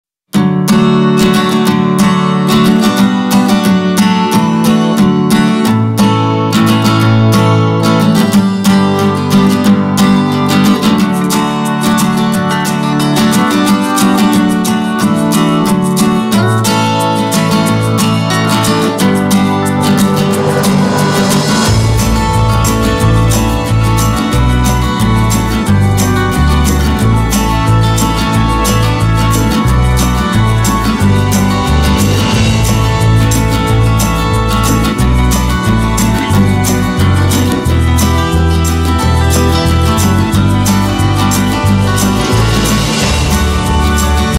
IPG fiber marking on aluminum
IPG fiber marking on coated metal,which used for high-end  name card with high precision,longer lifetime of the fiber generator,original German IPG fiber laser generator